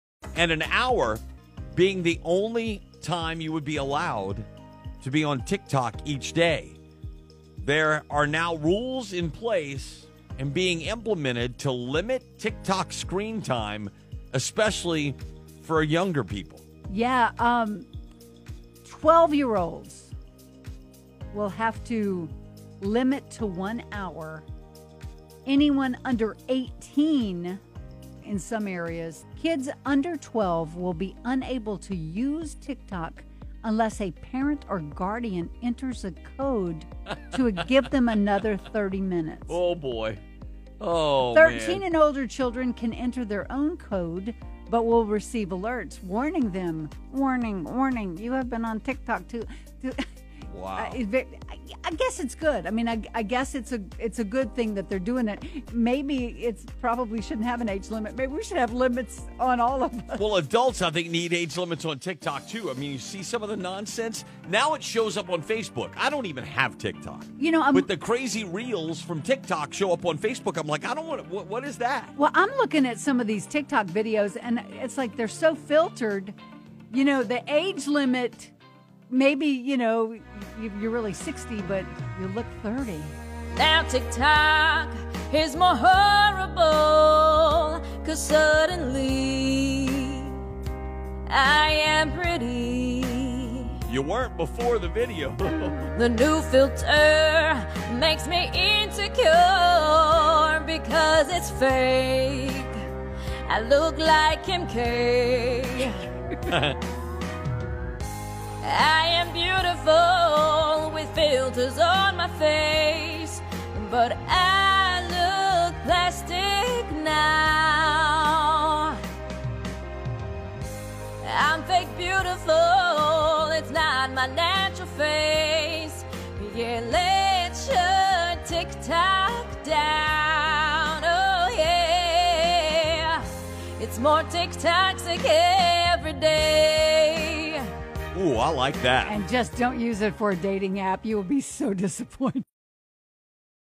TikTok Time Limit for Teens
TikTok users under 18 will now automatically have a 60-minute daily screen time limit.